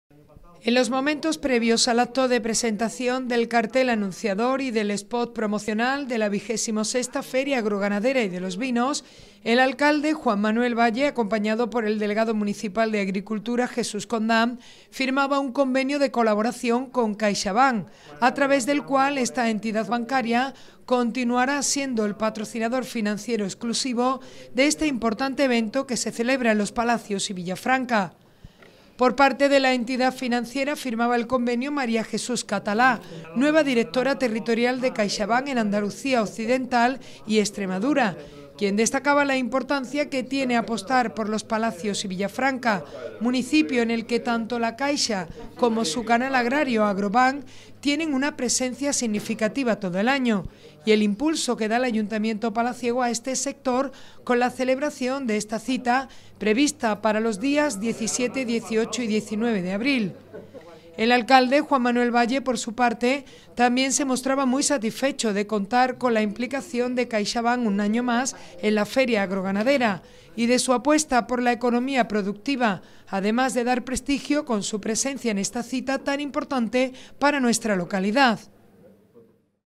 NOTICIA Firmado el convenio de patrocinio financiero exclusivo con Caixabank
PREVIO AL ACTO DE PRESENTACIÓN TUVO LUGAR LA FIRMA DEL CONVENIO DE COLABORACIÓN ENTRE AYUNTAMIENTO Y CAIXABANK, PATROCINADOR FINANCIERO EXCLUSIVO DE NUESTRA FERIA AGROGANADERA Y, ESTE AÑO 2020, DE LOS VINOS